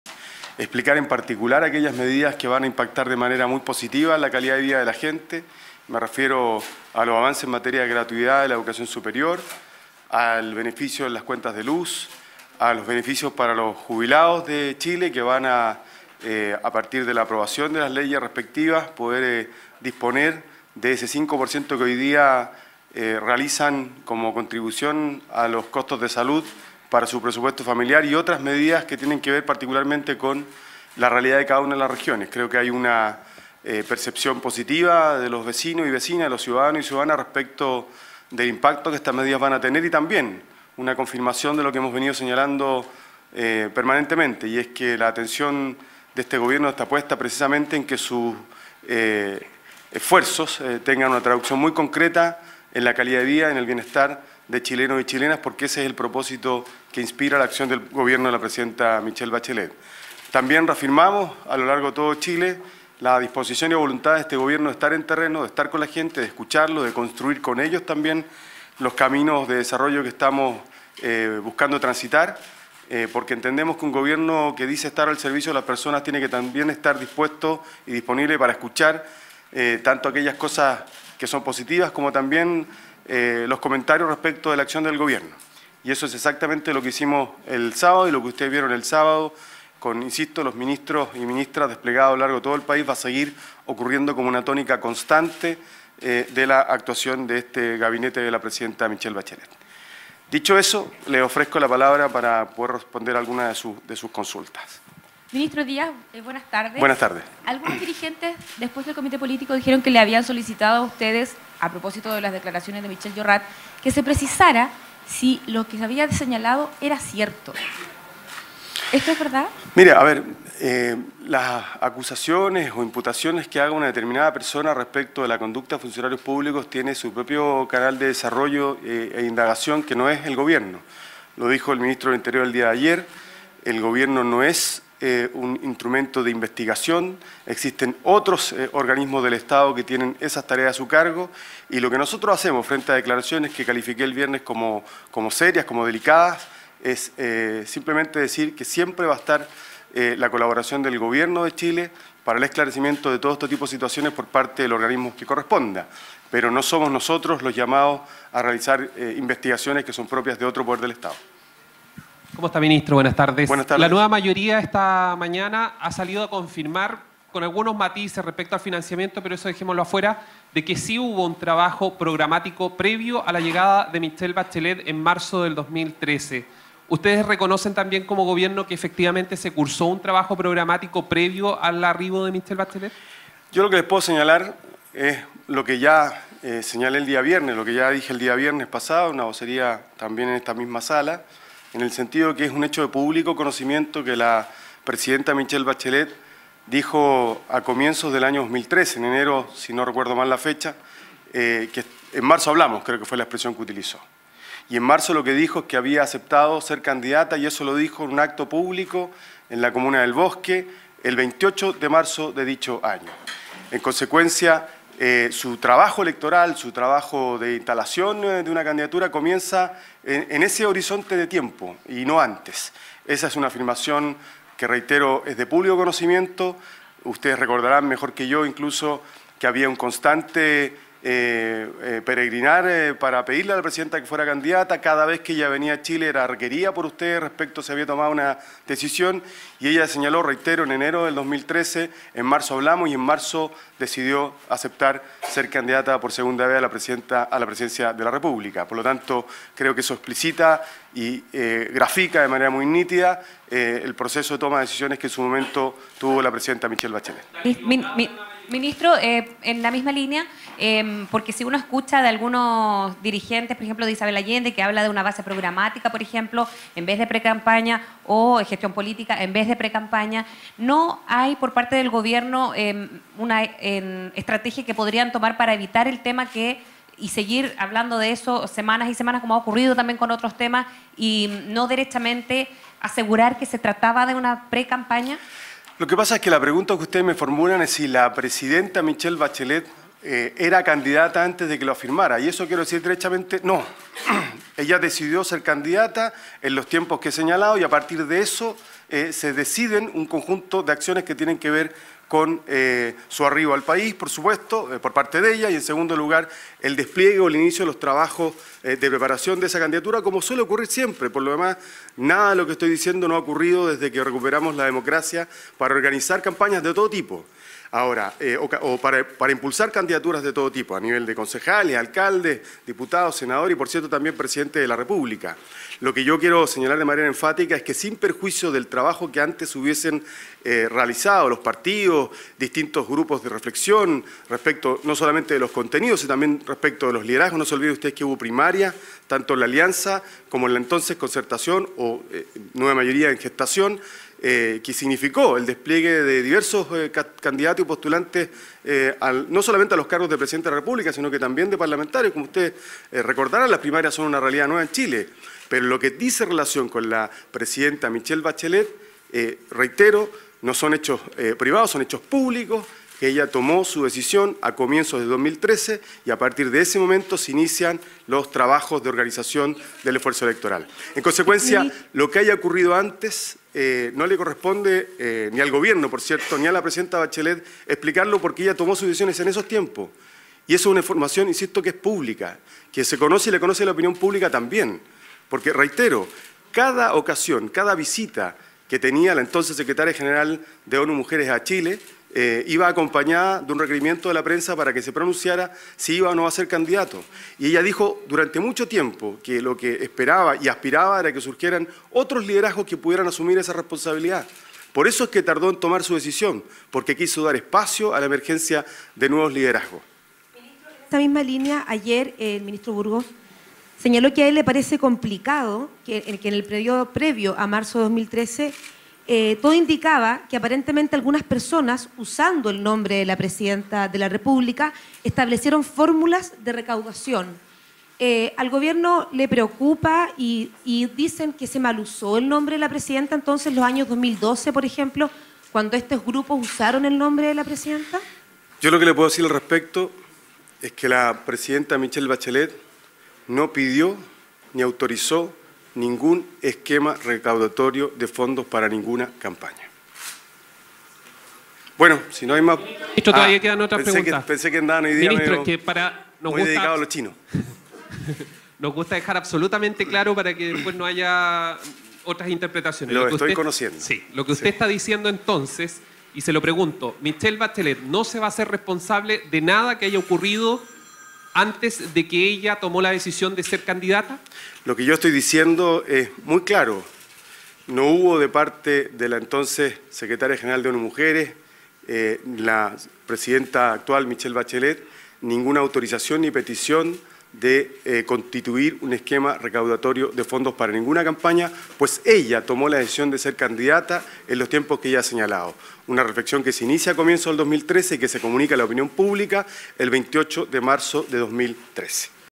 Gobierno descartó precampaña de Michelle Bachelet
La polémica se generó luego de que se conocieran pagos de la empresa de Martelli, a ex colaboradores de campaña de la mandataria.